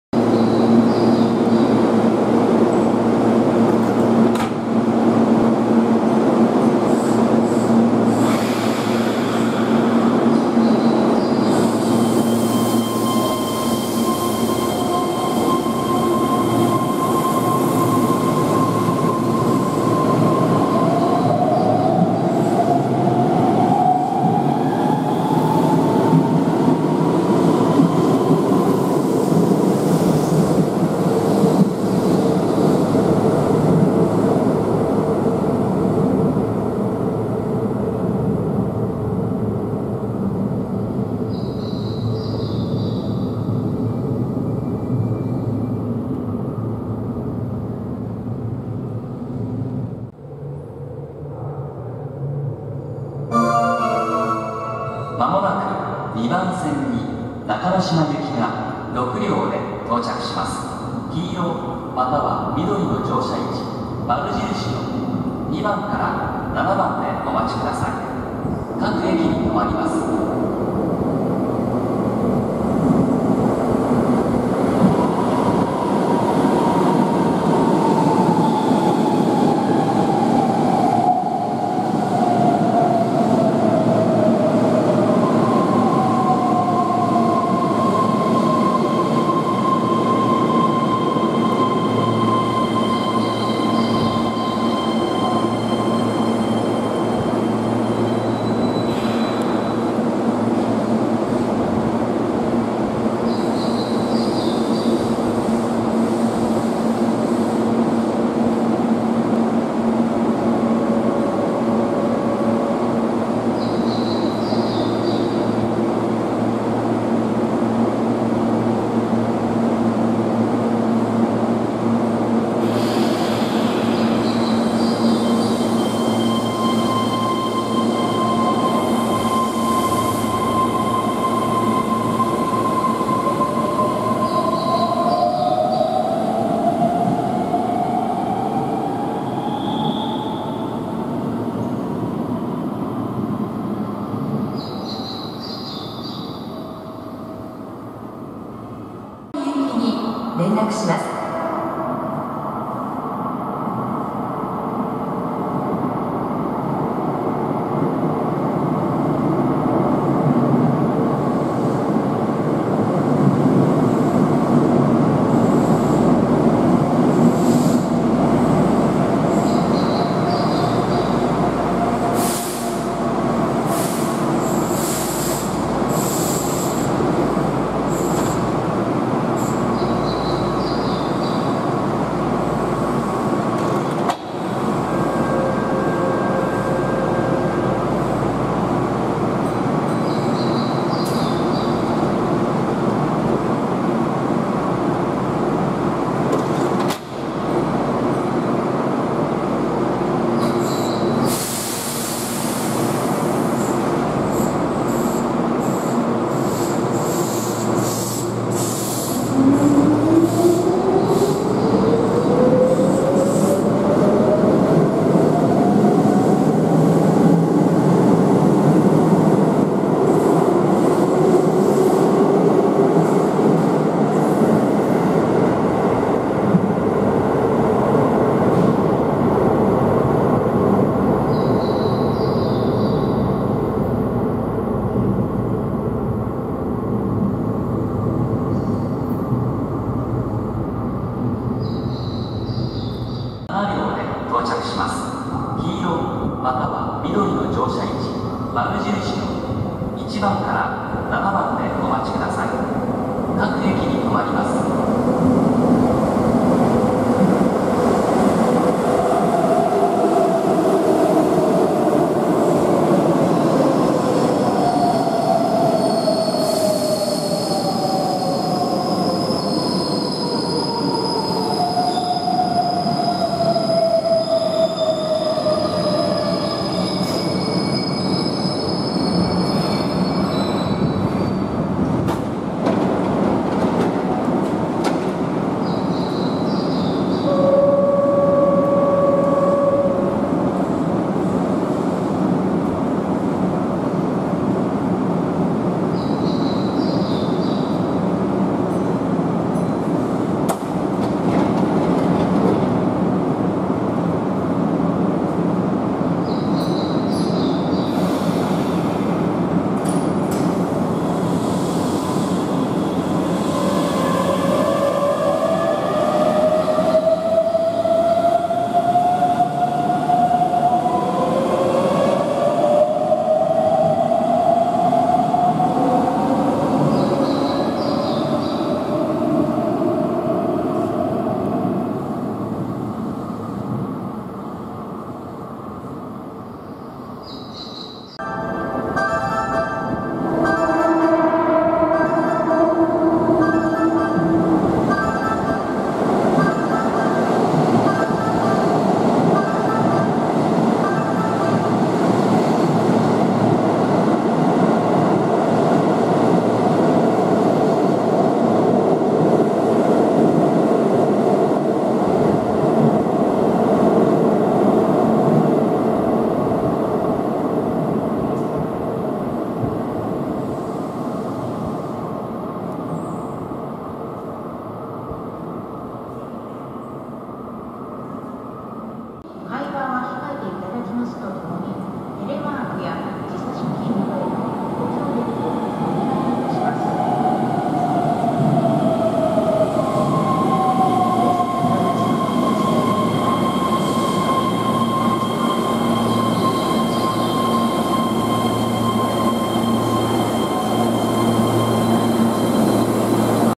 京阪大江橋駅 電車の発着・通過♪普通1000系と7200系、回送13000系【京阪中之島線/2022/07】
京阪中之島線「大江橋駅」の京阪電車（列車）の発着の様子です。回送のみ通過です。
昼に、普通（1000系、7200系、13000系）回送（13000系）を撮影しました。

00:00 普通(1.2番線)